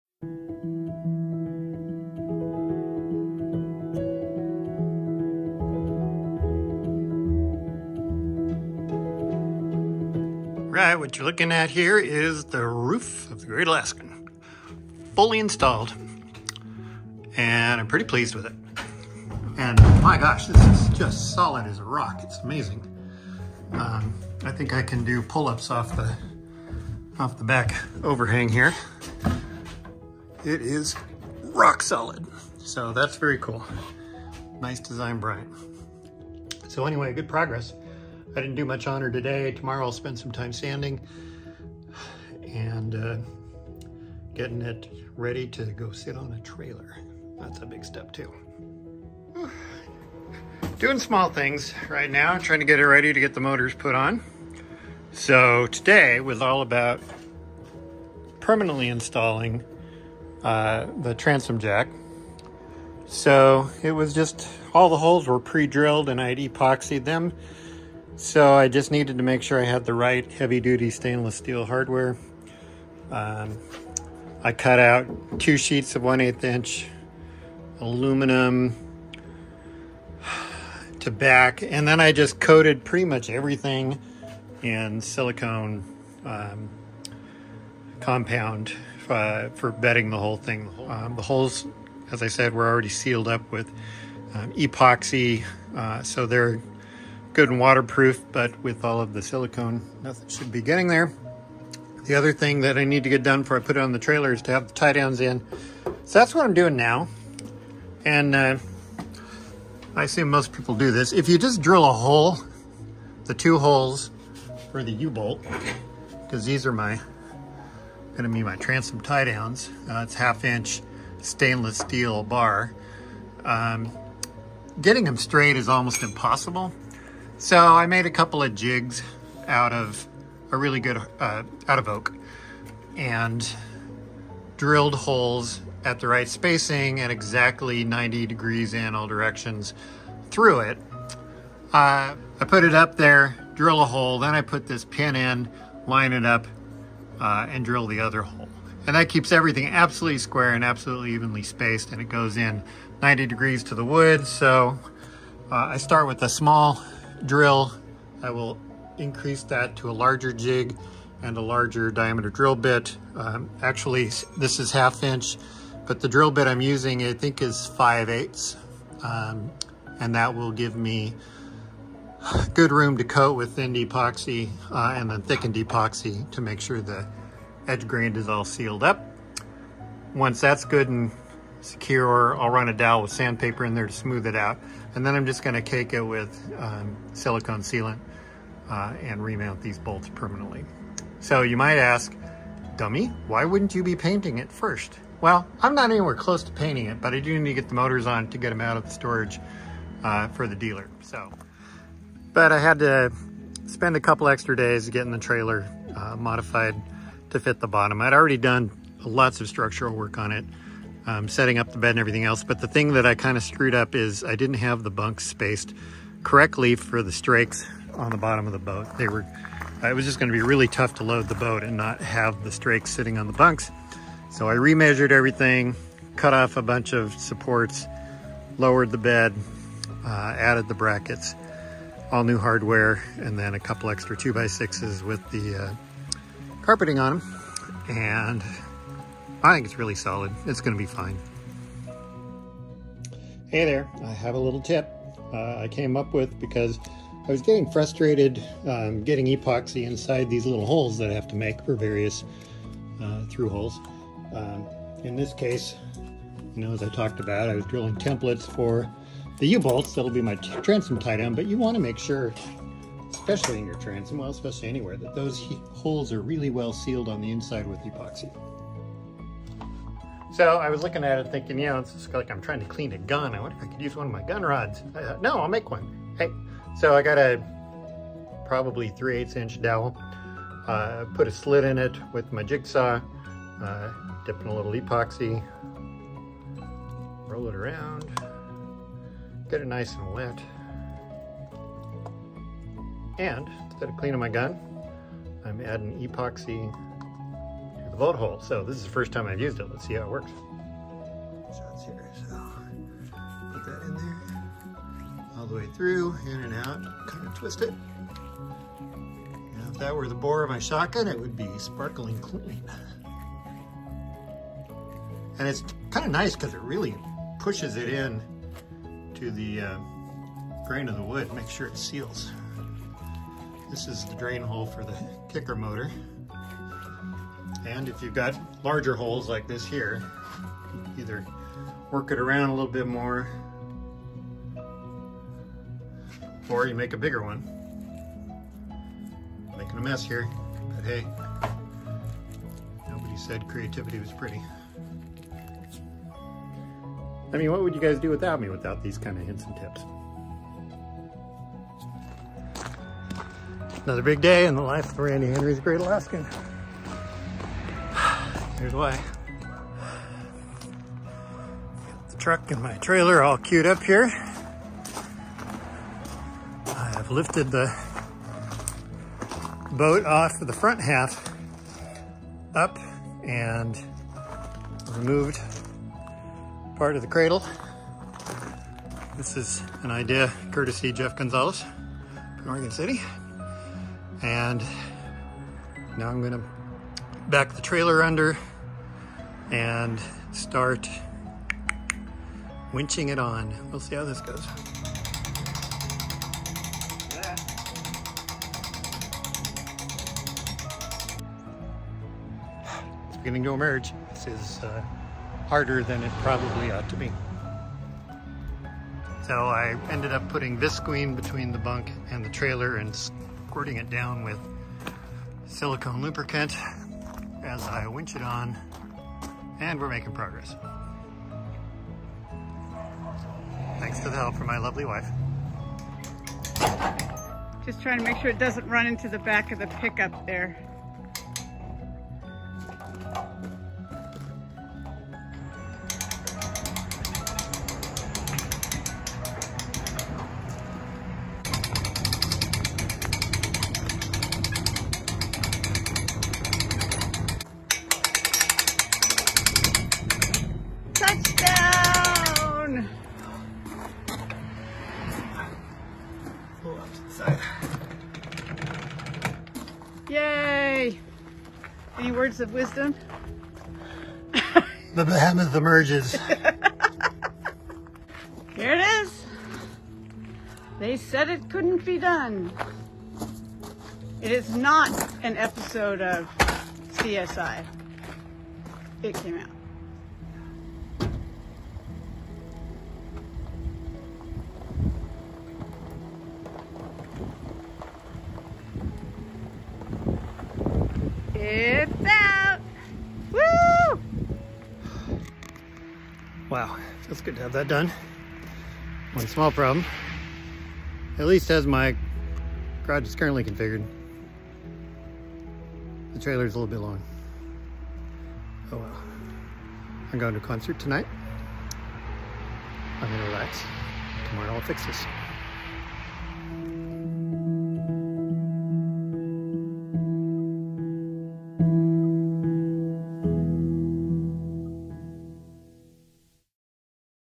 Corrected Version: Now It’s A Trailer Boat
Corrected version - Putting the Great Alaskan on the trailer.